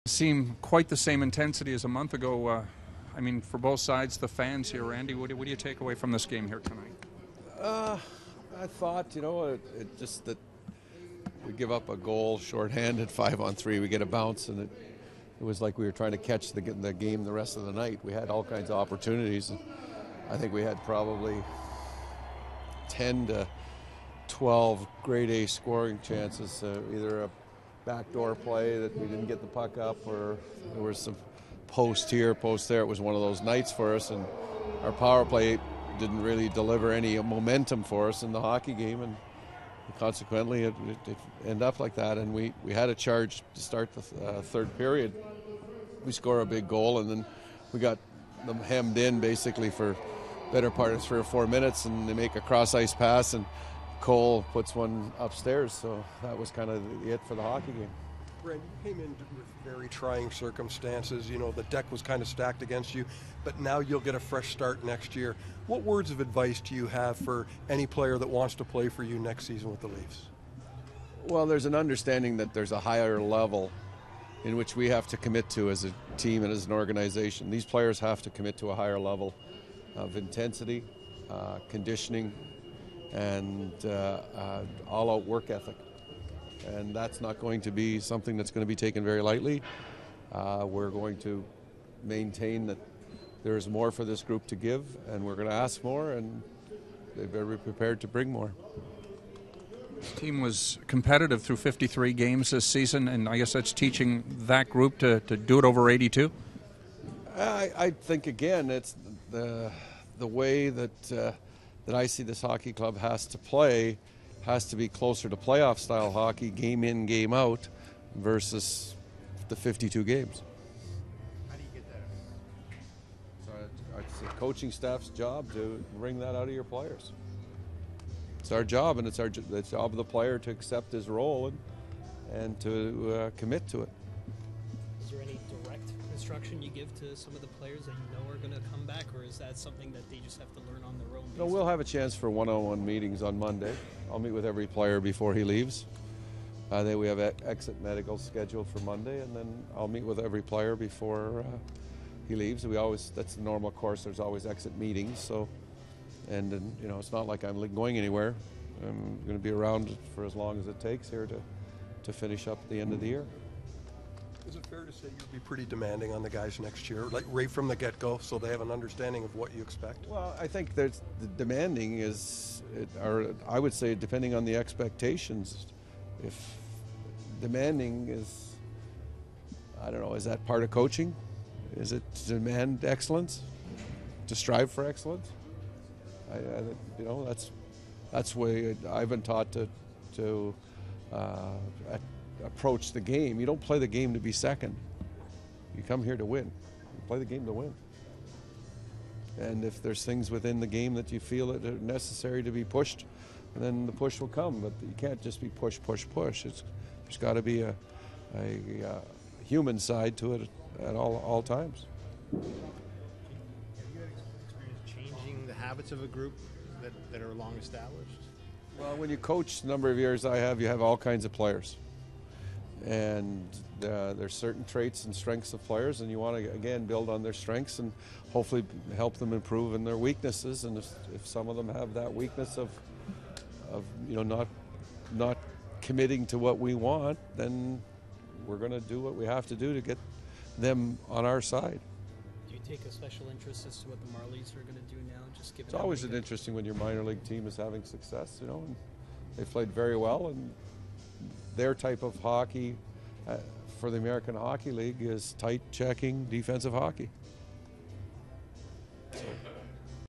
Randy Carlyle - 04/07/2012
Randy Carlyle addresses the media following the Leafs 4-1 loss to the Canadiens. For more post game reaction, go to mapleleafs.com